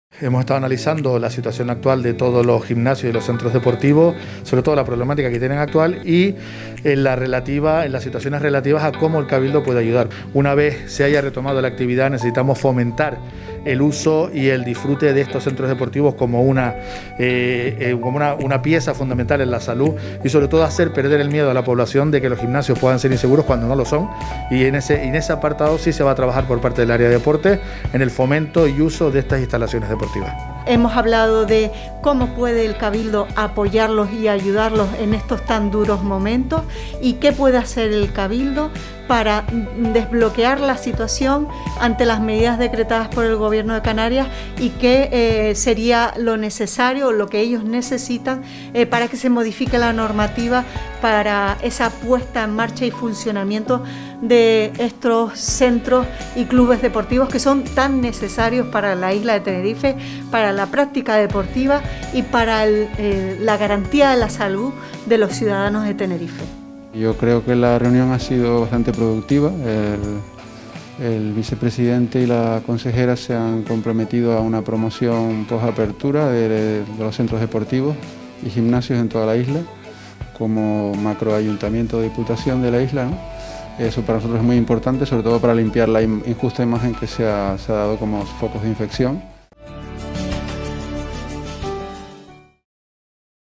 Apoyo a los centros deportivos privados
🏋‍♀🧘‍♂ GIMNASIOS: El vicepresidente del Cabildo, Enrique Arriaga, junto a la consejera insular de Deportes Tenerife, Concepción Rivero, ha manifestado el apoyo del Cabildo a los representantes de los gimnasios de la isla por el cierre de estos durante las restricciones impuestas en el nivel 3 de alerta causado por la pandemia.

Por ello, el vicepresidente les ha propuesto la realización de campañas para promocionar los centros deportivos privados como lugares seguros y esenciales para la promoción de la salud.